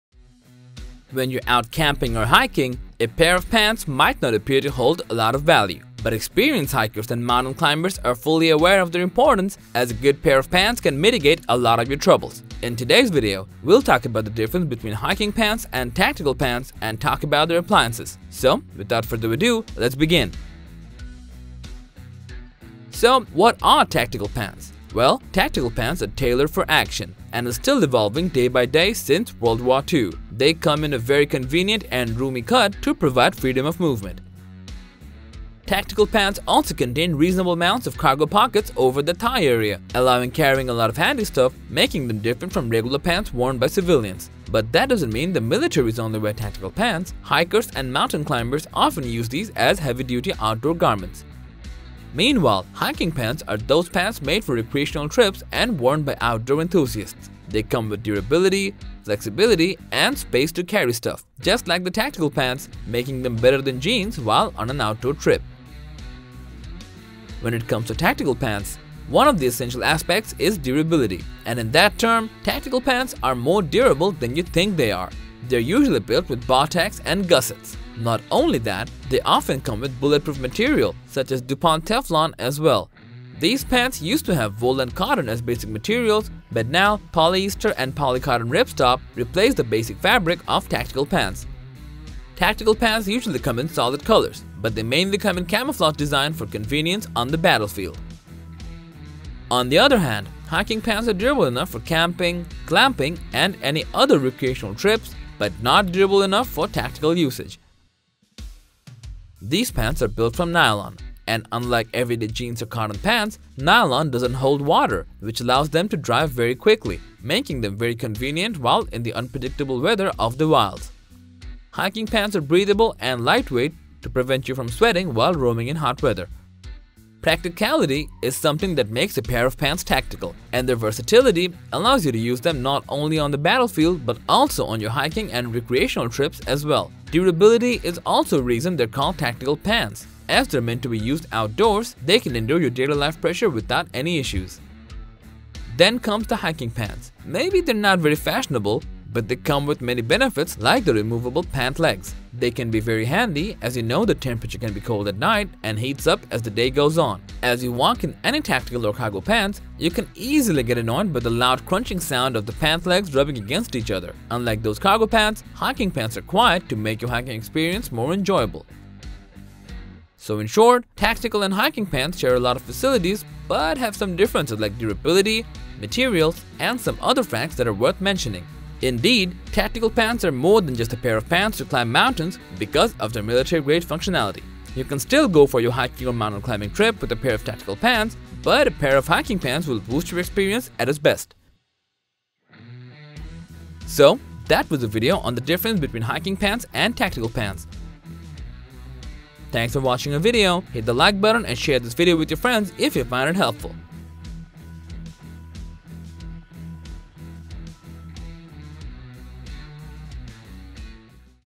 Hiking Pants vs Tactical Pants - What's the Difference?
Hiking Pants vs Tactical Pants- we have tried to shed some light on the topic. Often it's easy to get confused with hiking pants and tactical pants as they are almost similar in terms of design and usage. But are they actually the same? Our today's video is going to be about Hiking Pants vs Tactical Pants.